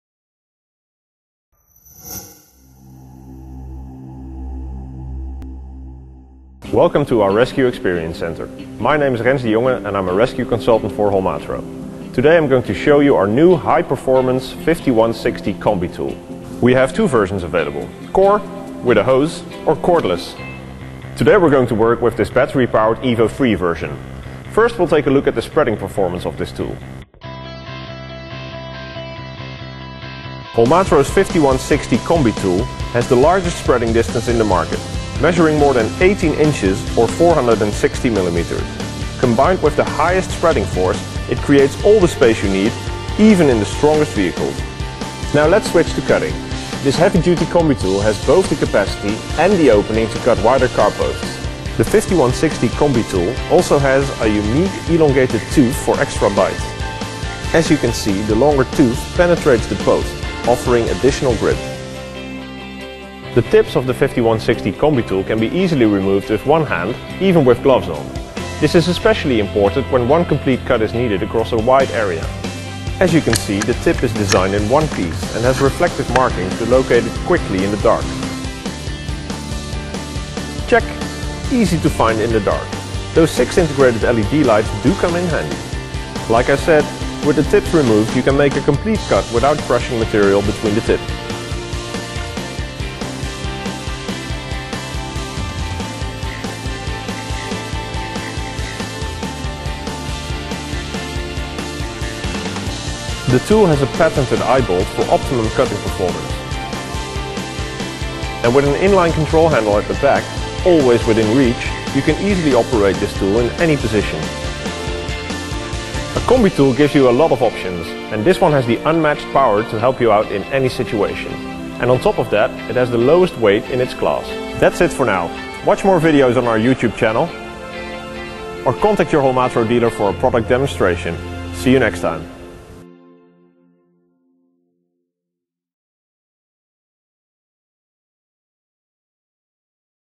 NUEVA Herramienta Combinada CT5160 - Holmatro
Lanzamiento de Holmatro.
Esta herramienta tiene una gran apertura de cuchillas de casi 40 cms, una fuerza de corte de 94.7 toneladas es decir casi 3 veces que su predecesora la 4150.
Está disponible tanto en la versión con Tecnología CORE como EVO3 a batería.

Impleseg- distribuidor autorizado para Colombia